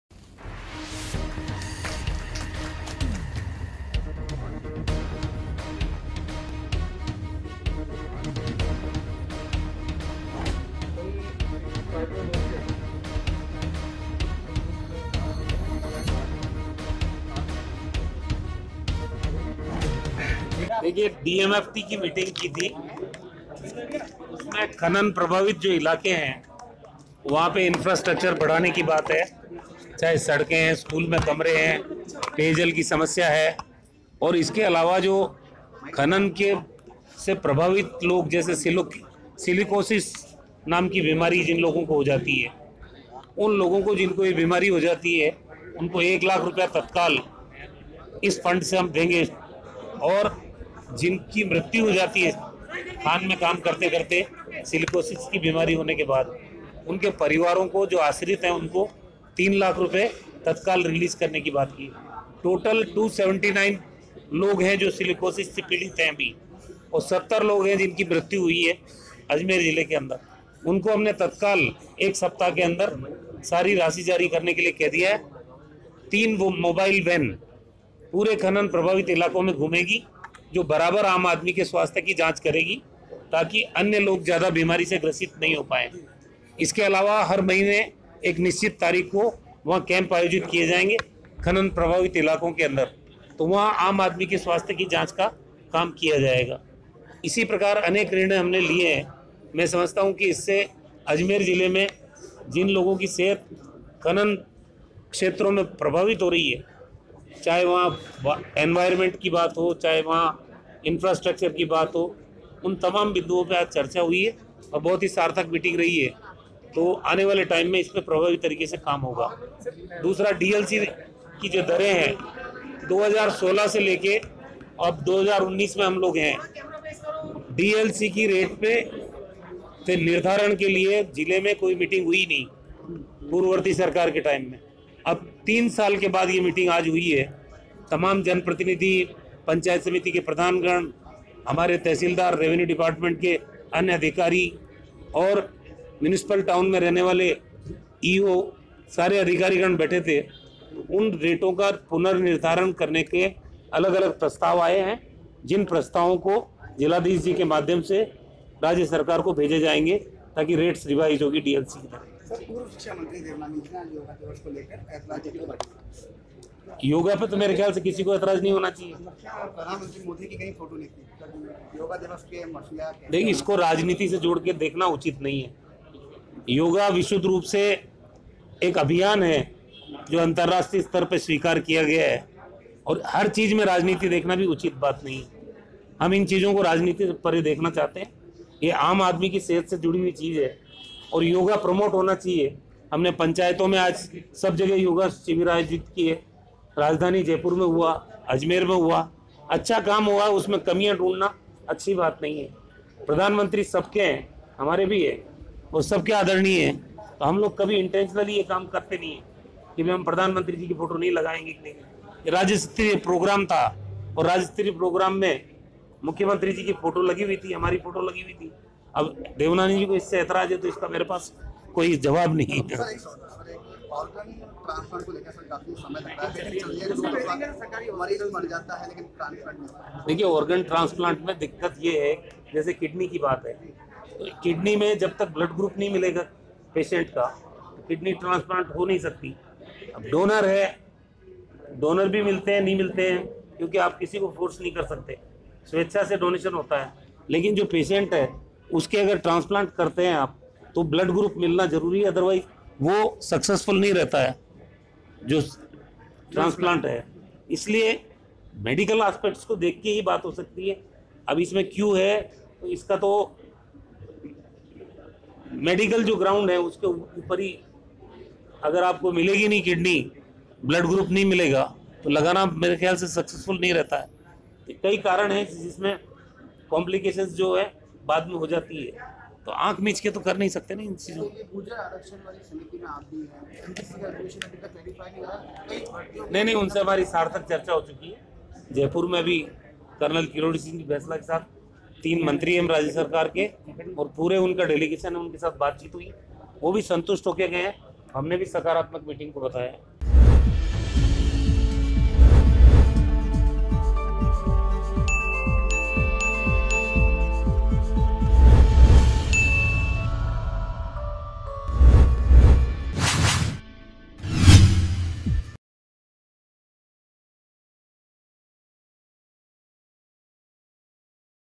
AJMER NEWS | जिला कलक्ट्रेट में चिकित्सा मंत्री रघु शर्मा की अध्यक्षता में बैठक | MTTV INDIA
जिला कलक्ट्रेट में चिकित्सा मंत्री रघु शर्मा की अध्यक्षता में बैठक,खनन और डीएलसी की बैठक का आयोजन,जिला कलेक्टर विश्वमोहन शर्मा के साथ अधिकारी मौजूद